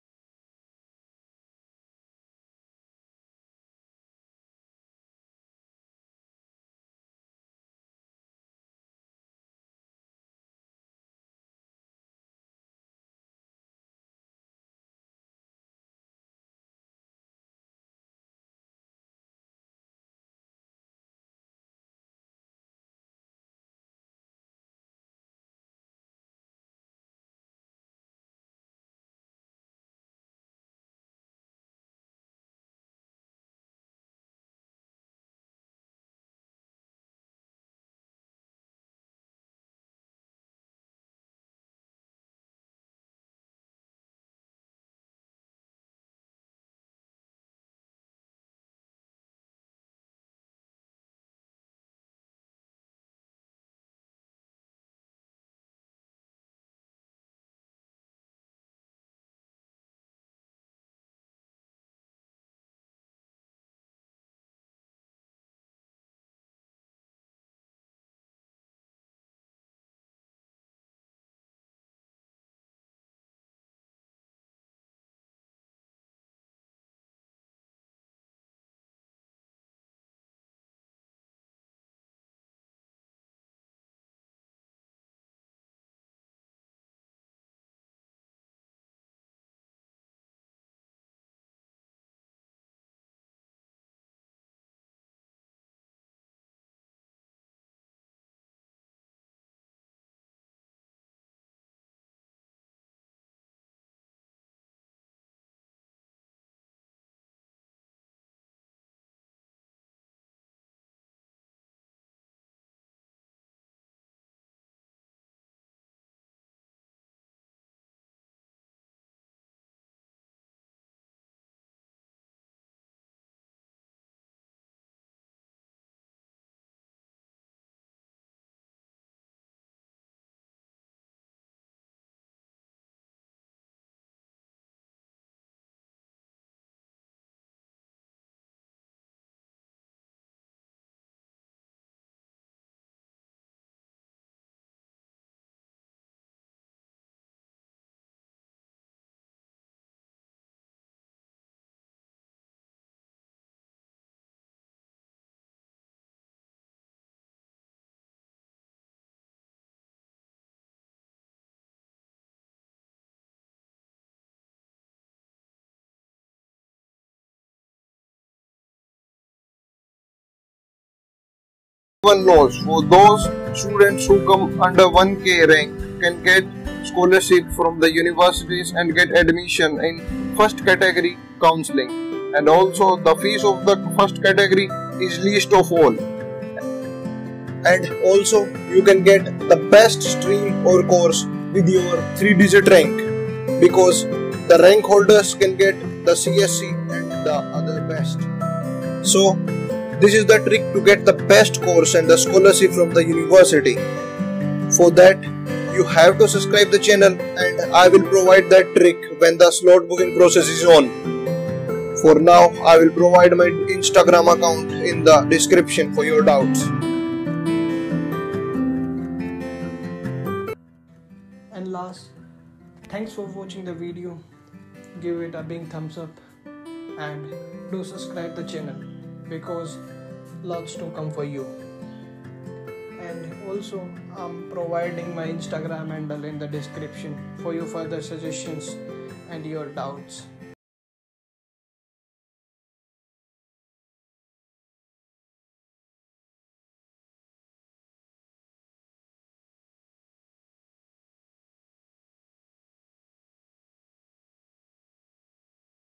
VIT SLOT BOOKING , VITEEE , UNDER 1K RANK , CSE .
SLOT BOOKING , VITEEE , UNDER 1K RANK , CSE . All about vit . vit is 13 ranked nirf college and vit slot booking with all information of booking and a trick to get under 1k ranking exam and to get CSE branch . how to get admission in 1 category . Also why people choose vit and about the placements and campus of the university.